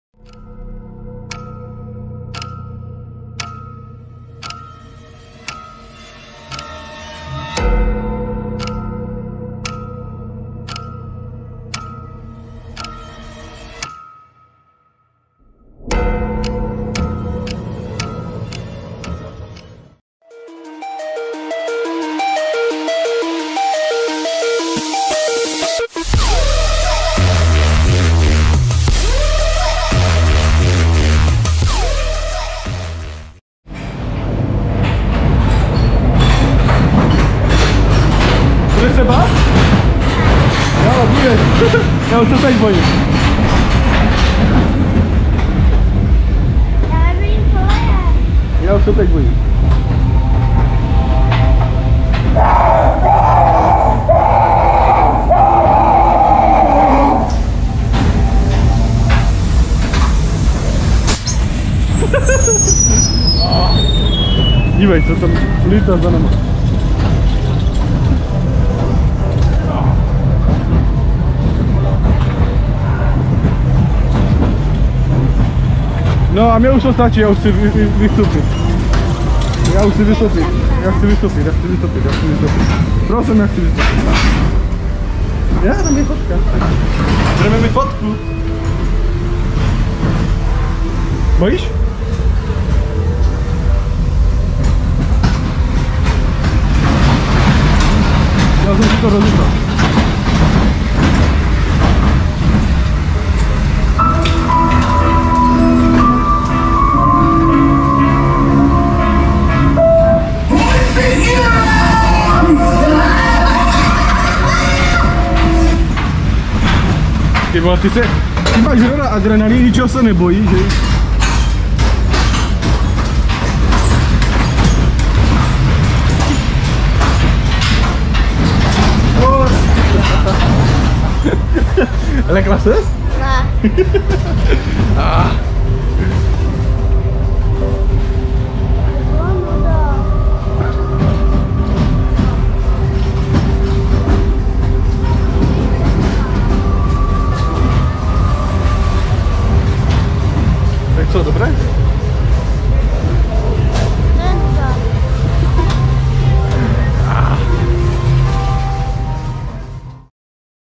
Matějská pouť 2024 - Ghost villa za-jízdy
cena pro jednoho: 150Kč provozovatel: C. Sipkema
tato akce probíhá od 24.2. do 14.4.
natáčeno Praha - Holešovice dne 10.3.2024